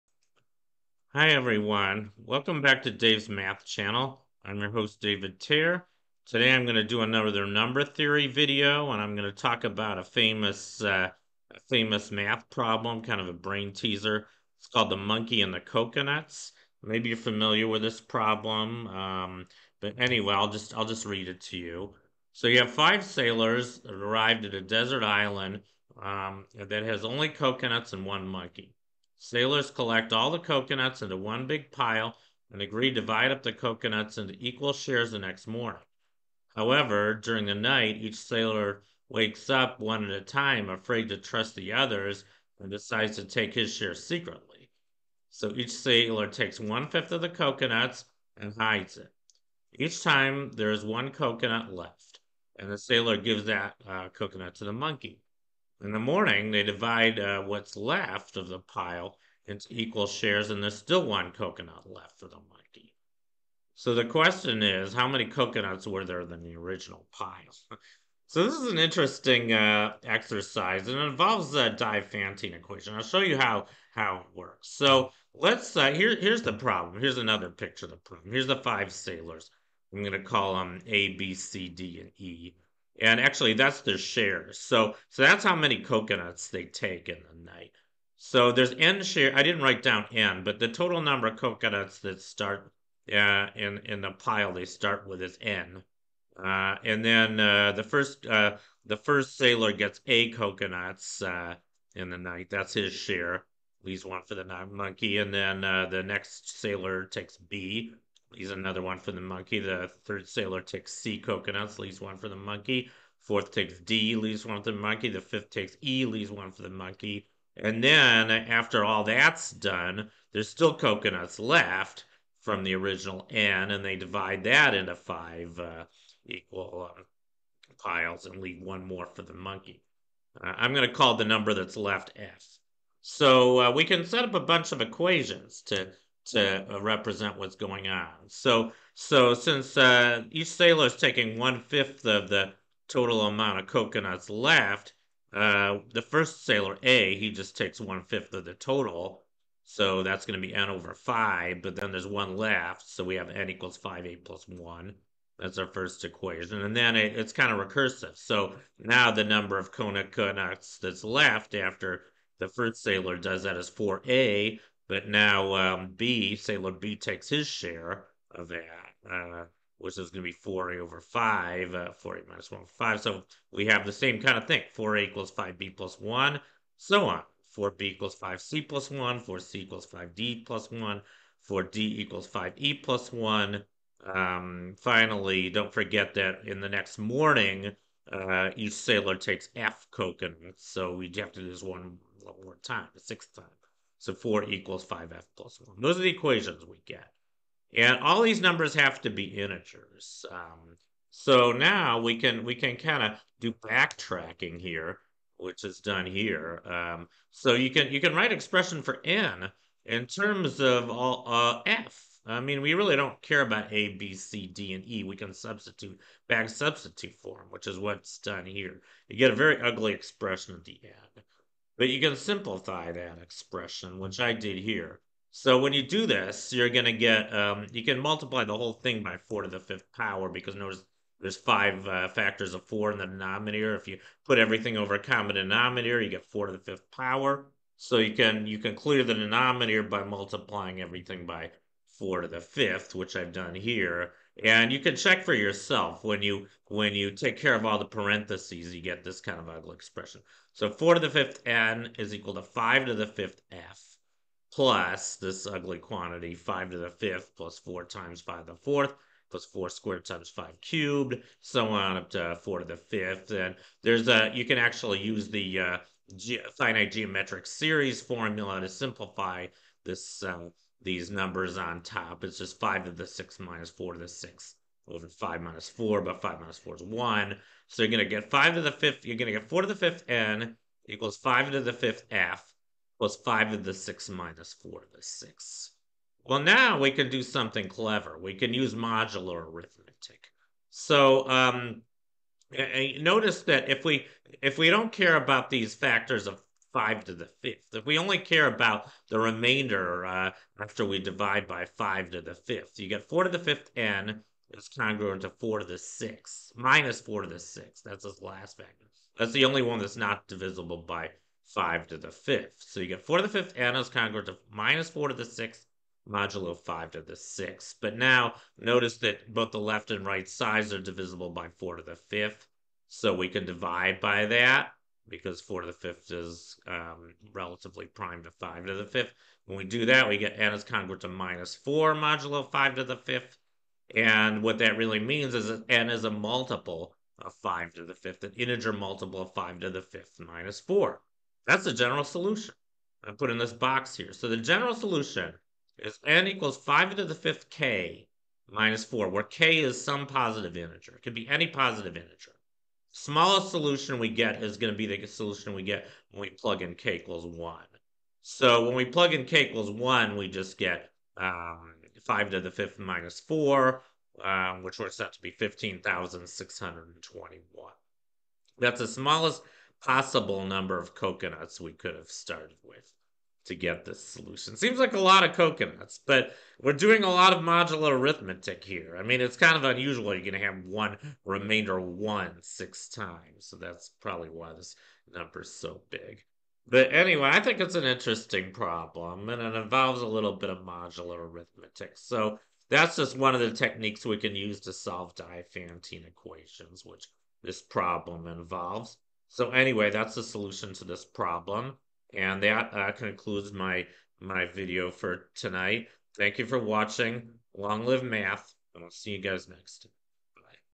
The Monkey and the Coconuts
In this video, I show how modular arithmetic can be used to solve a famous old math problem called The Monkey and the Coconuts.